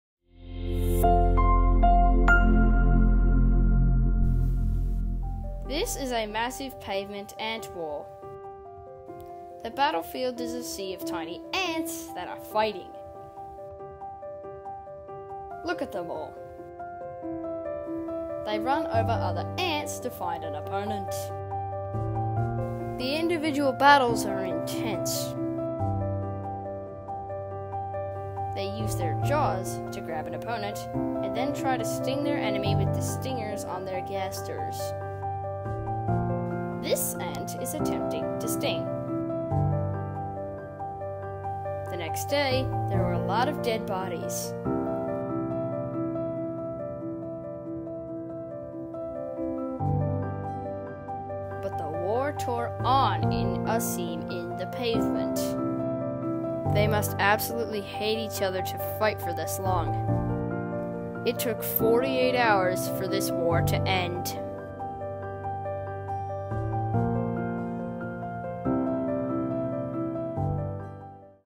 Pavement Ant War
A battle between two colonies of pavement ants